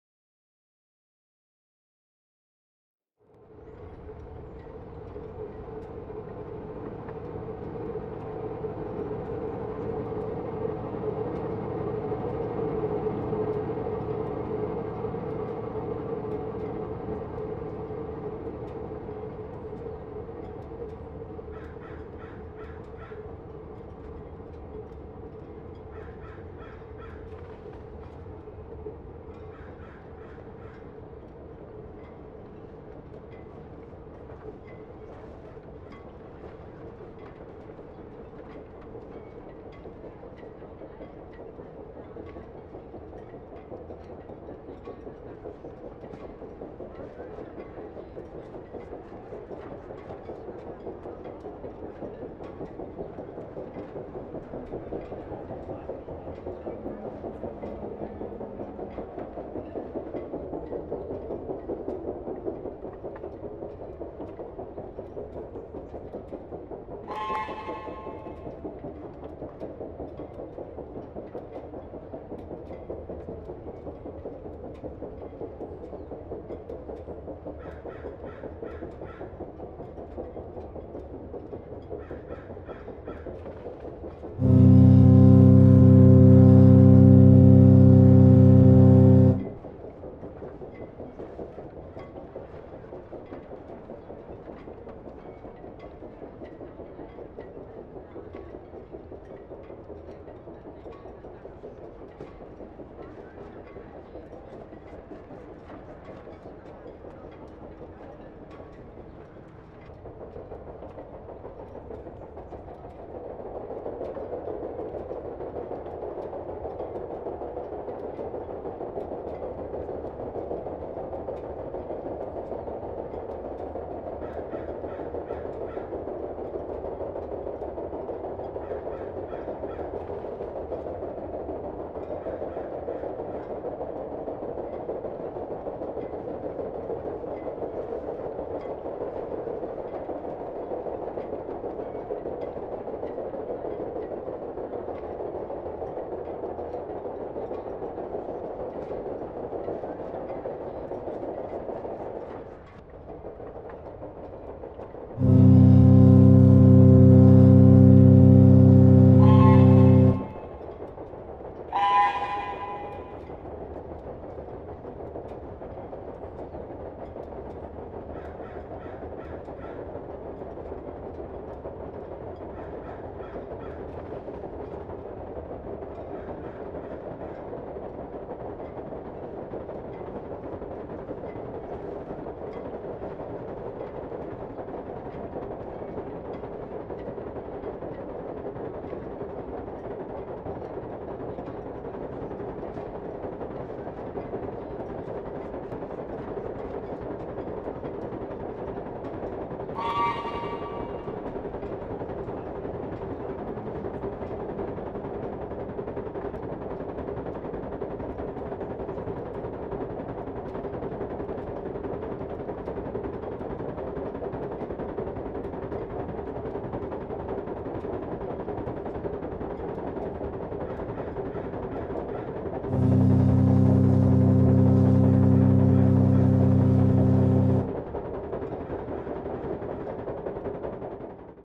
Lusitania leaving New York
This is the final video of the RMS Lusitania leaving New York docks for her faithful voyage on May 1st 1915.

ships horn.WAV by inchadney
steam train horn 02 by eliasheuninck
synth_steam_engine.wav by roccapl
Synthetic Steam Engine by udicangiotti
motor industrial turbine spinning.flac by kyles
Harbor Ambience 1 by cliftonmcarlson